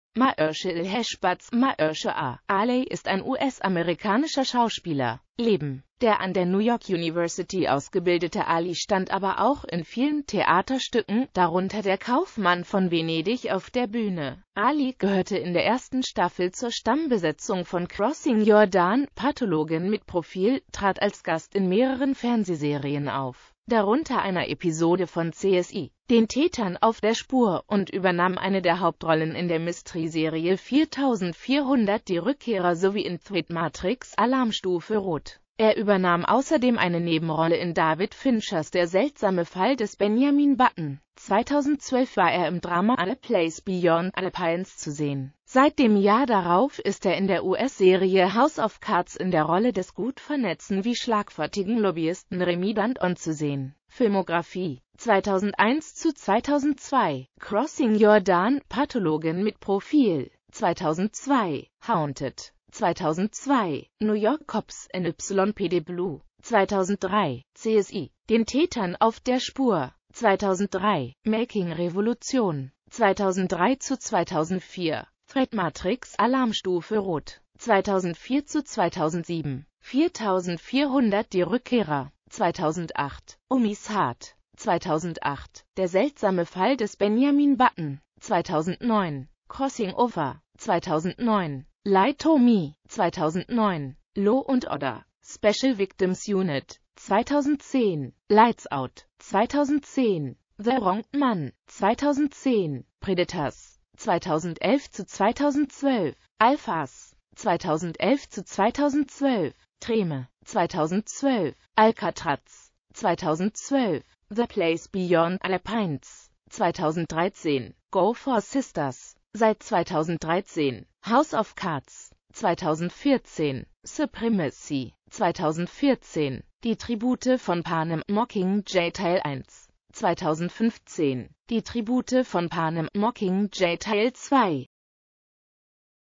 Mahershalalhashbaz Ali
Vielen Dank für Ihre Unterstützung:

Mahershalalhashbaz „Mahershala“ Ali (* 16.Februar 1974 in Oakland, Kalifornien als Mahershalalhashbaz Gilmore) ist ein US-amerikanischer Schauspieler.